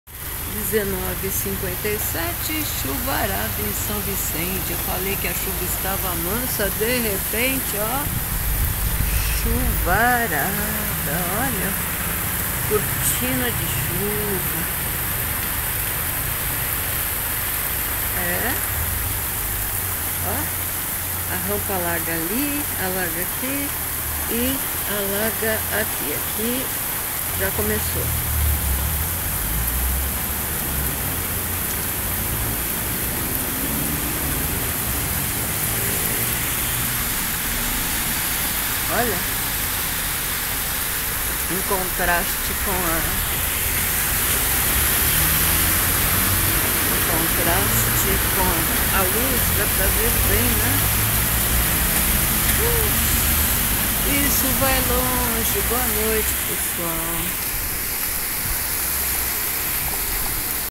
Pancadão de chuva agora pouco em São Vicente - litoral de SP
Agora na edição já diminuiu, mas acho que não vai parar não. Tem gente que sofre sempre com as chuvas, há anos, em alguns lugares. Aqui na região ainda há locais que alagam até por causa das marés.